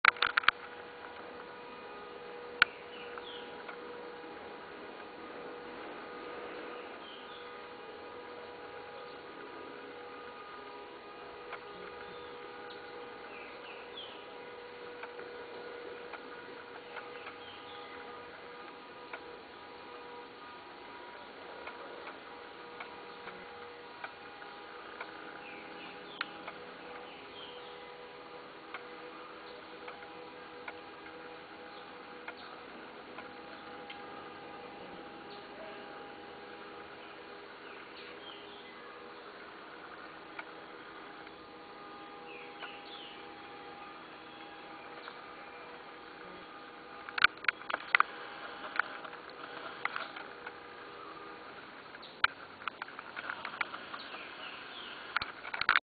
Borboleta come banana.
Duas espécies diferentes de borboletas dividem a comida: banana